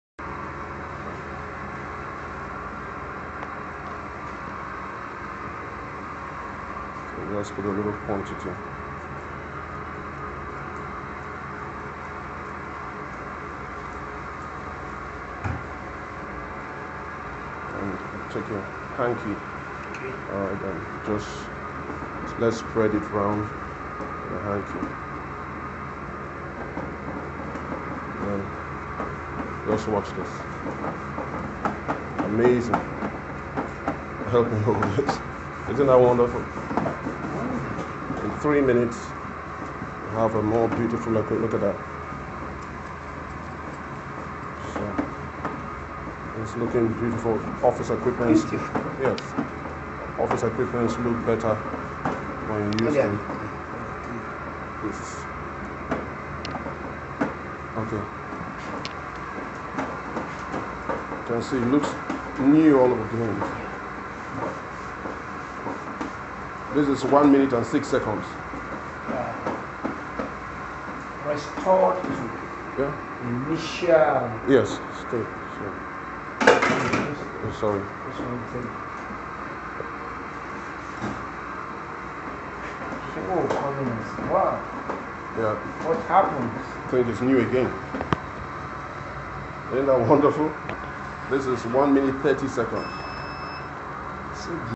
RESTORE OFFICE EQUIPMENTS FASTER,CHEAPER,SAFER WITH MAFITO MULTIPURPOSE SUPER CLEAN
ONE CLEANING AGENT , MANY FUNCTIONS, GOOD WITH GOLD, SILVER, LEATHER, ALUMINIUM, HEADLAMPS, TILES, CHROME, GLASS, RADIATOR, AIR CONDITIONERS, PERFUMED, HAND FRIENDLY ETC. ,